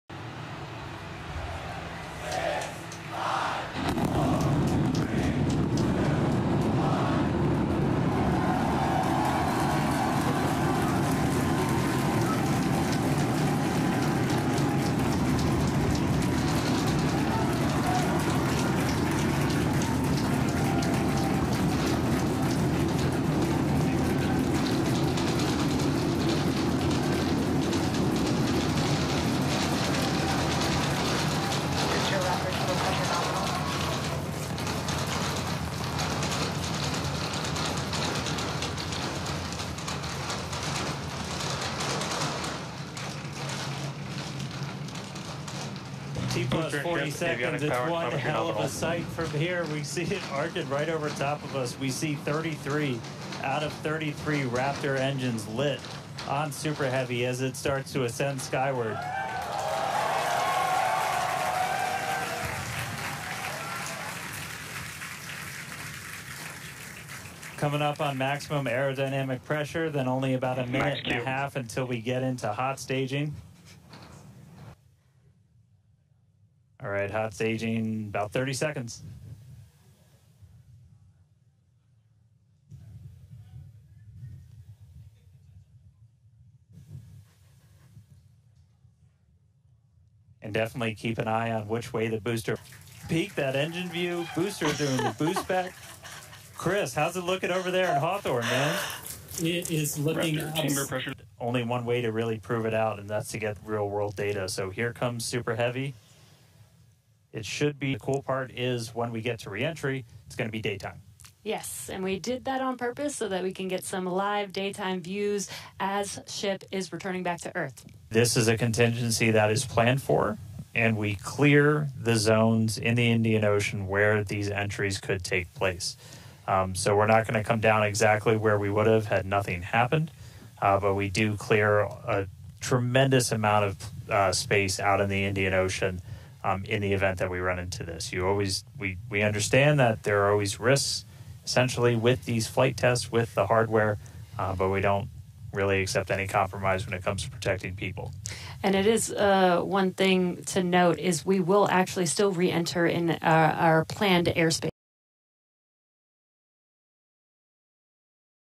SpaceX's Starship spins out of control after flying past points of previous failures
SpaceX's Starship rocket roared into space from Texas on Tuesday (May 27) but spun out of control about halfway through its flight without achieving some of its most important testing goals, bringing fresh engineering hurdles to CEO ElonMusk's increasingly turbulent Mars rocket program.